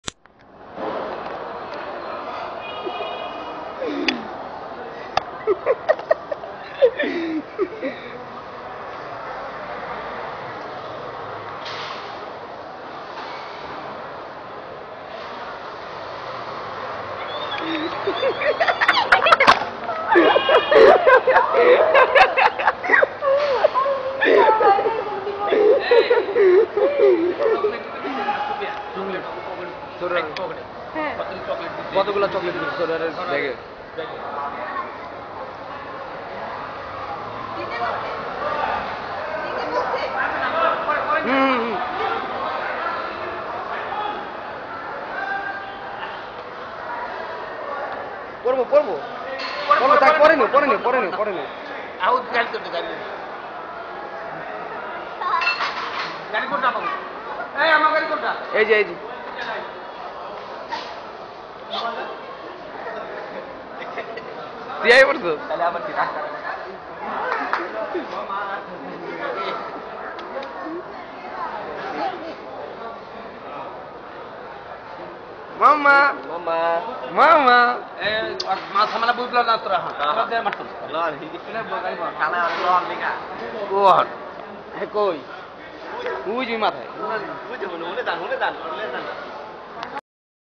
First time back to Bangladesh,injoy
first time caome to bangladesh from itali,then she so excited.